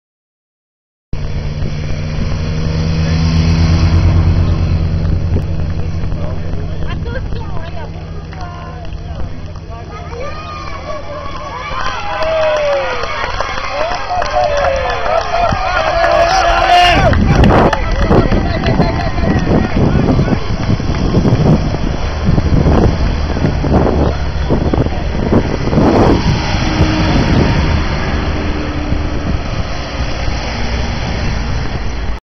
Tour de France 2009 dans La Côte de Prévonloup (8/8)
Le dernier coureur avant la voiture balais. Il s'agit du Hollandais Kenny Robert Van Hummel.